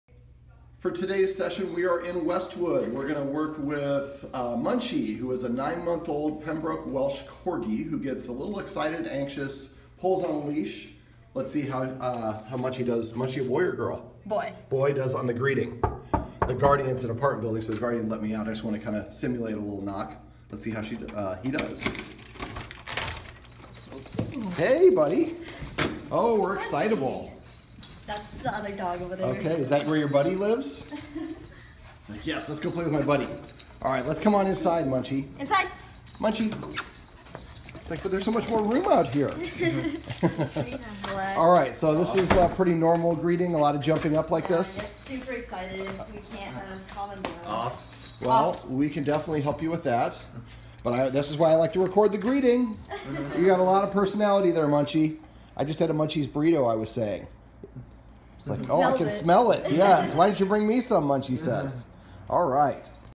Meeting Munchie
Munchie is a nine-month-old Pembroke Welsh Corgi who lives in Westwood in LA. His guardians set up a dog behavior training session with me to stop him from barking at things he doesn't like or understand.